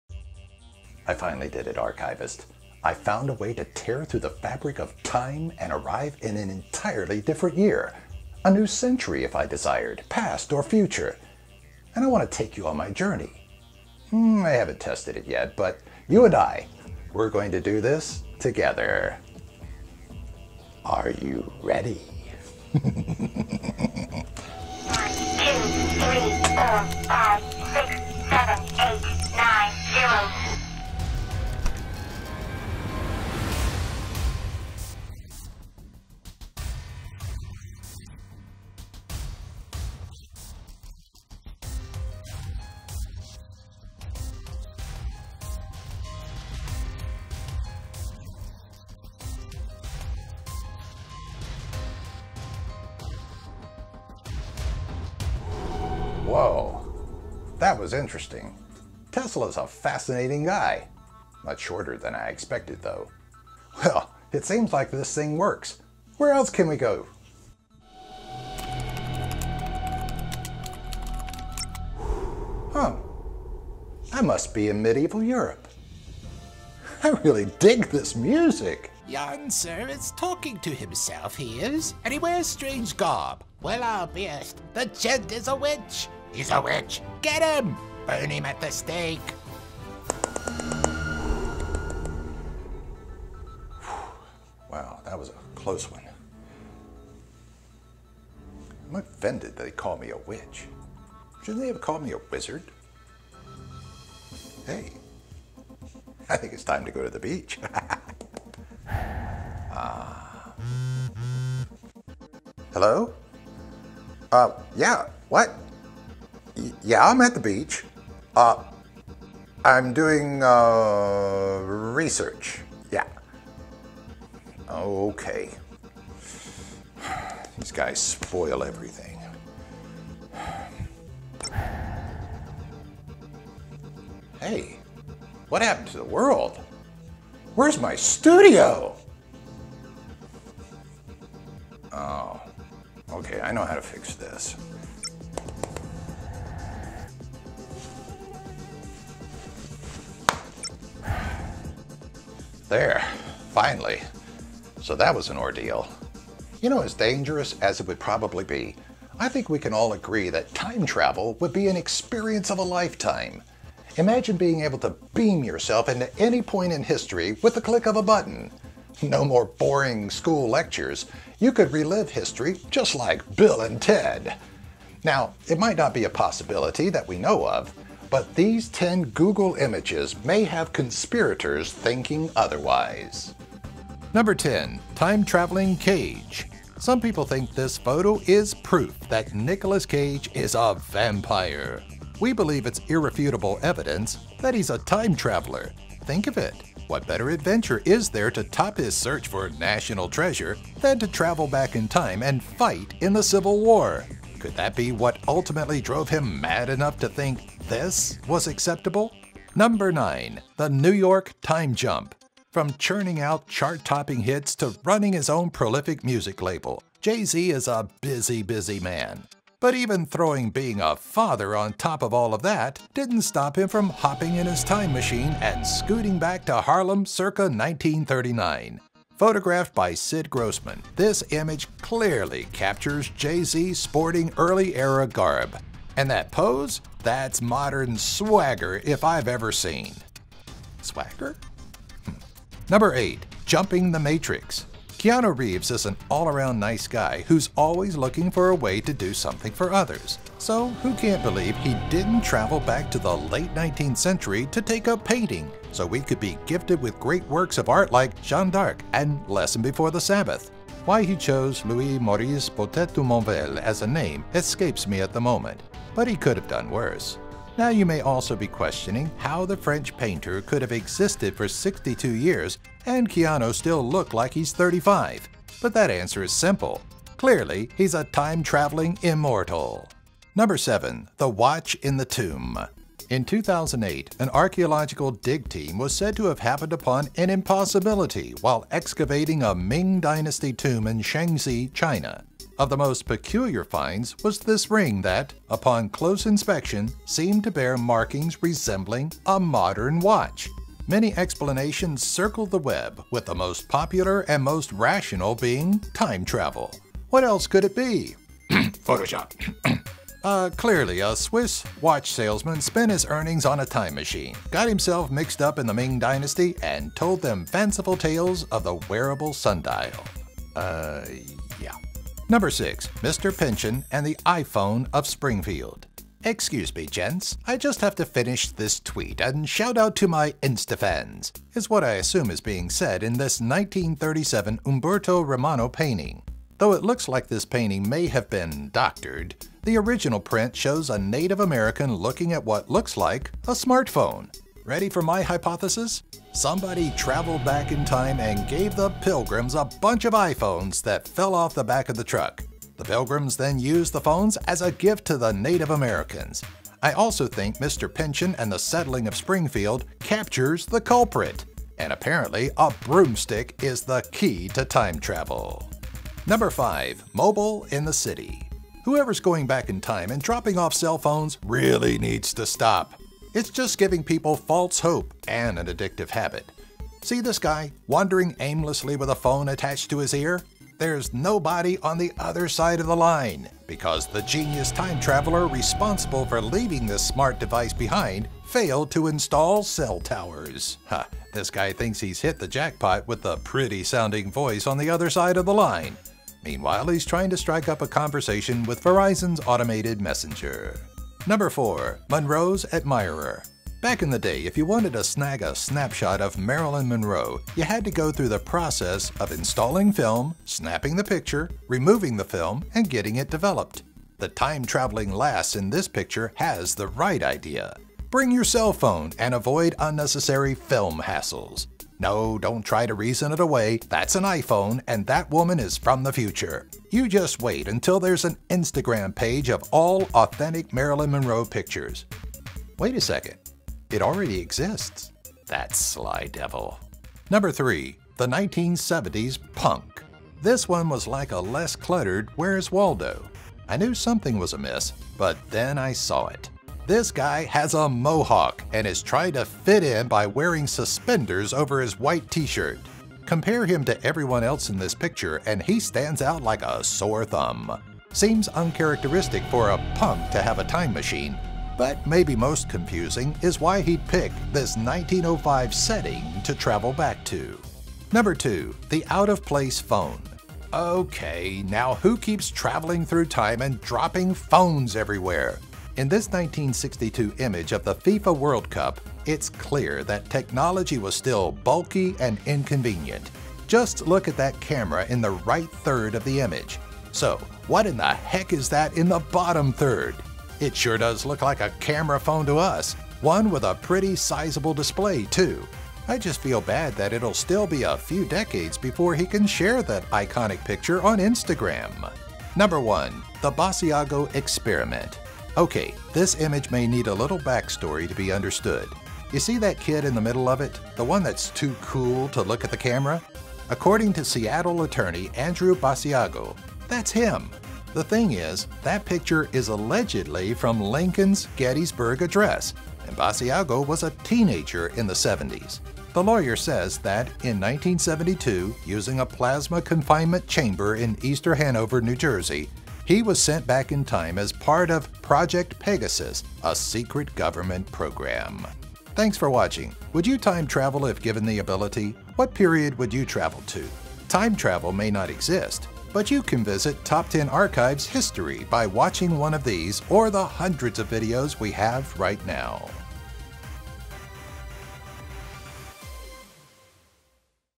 Top 10 Google Images That Prove TIME TRAVEL Exists
Time travel might not be a possibility that we know of, but these 10 Google Images may have conspirators thinking otherwise..

10. Time Traveling Cage
9. The New York Time Jump
8. Jumping the Matrix
7. The Watch in the Tomb
6. Mr. Pynchon and the iPhone of Springfield
5. Mobile in the City
4. Monroe’s Admirer
3. The 1970s Punk
2. The Out of Place Phone
1. The Basiago Experiment